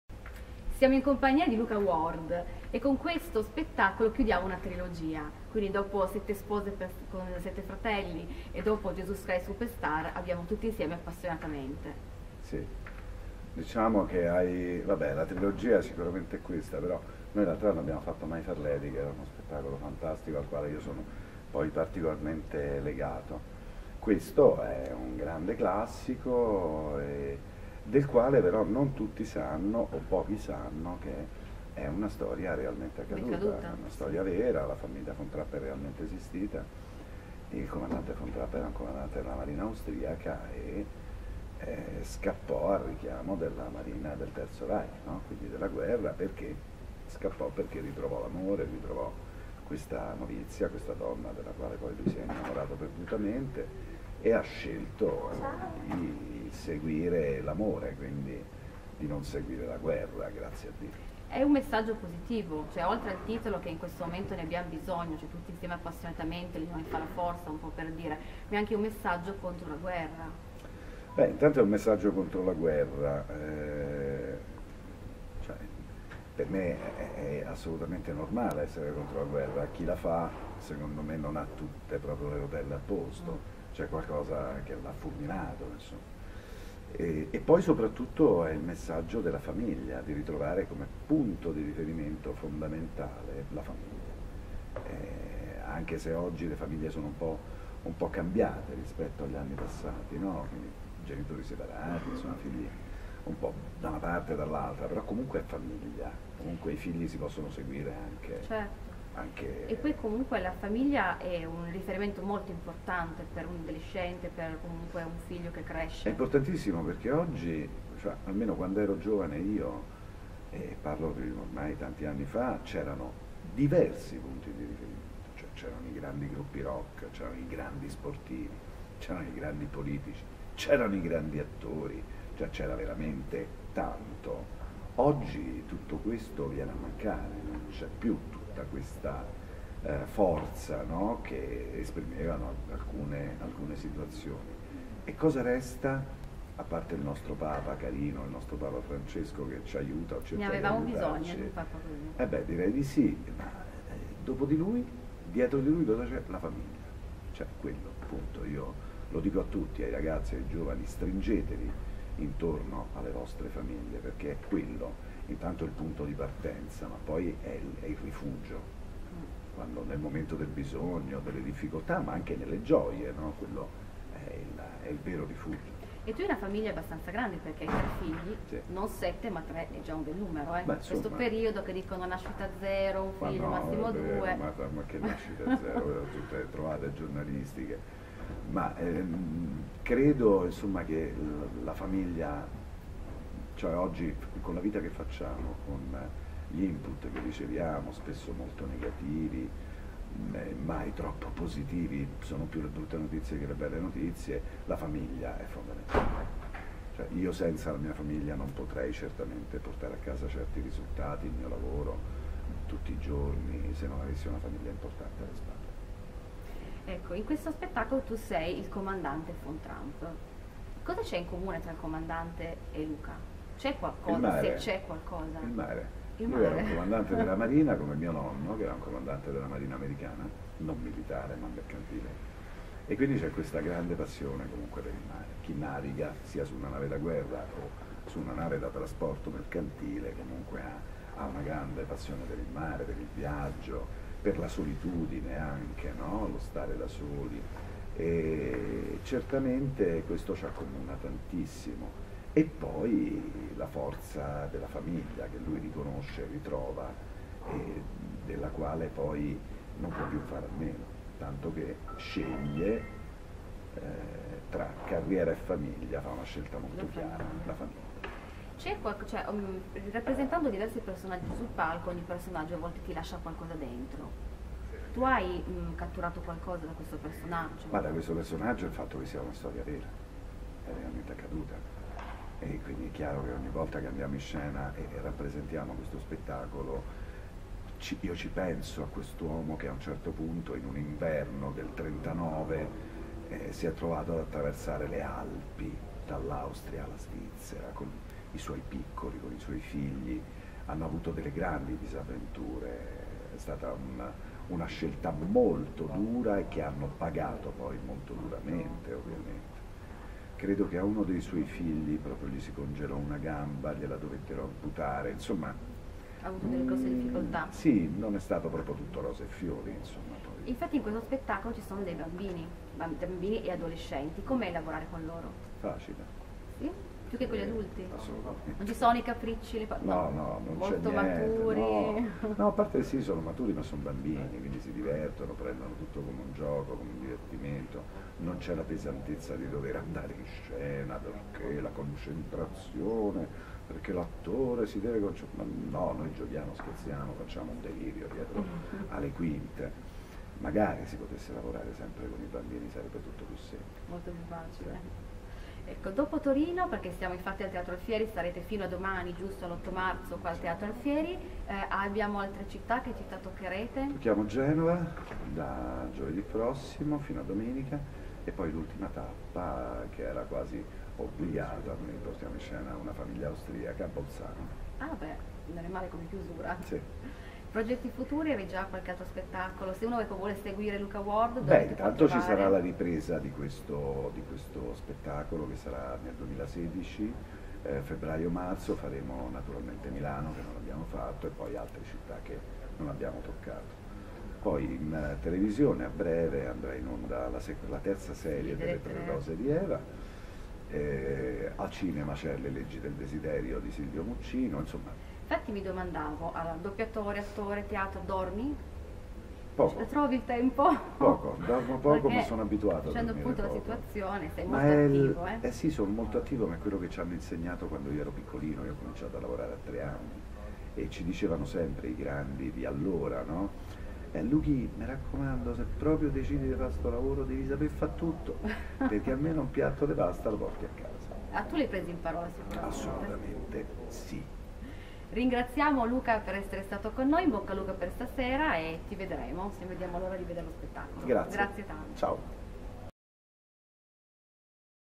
Tutti insieme appassionatamente: intervista a Luca Ward
Luca Ward racconta a Donneingamba TUTTI INSIEME APPASSIONATAMENTE. Su troverai le foto dello spettacolo.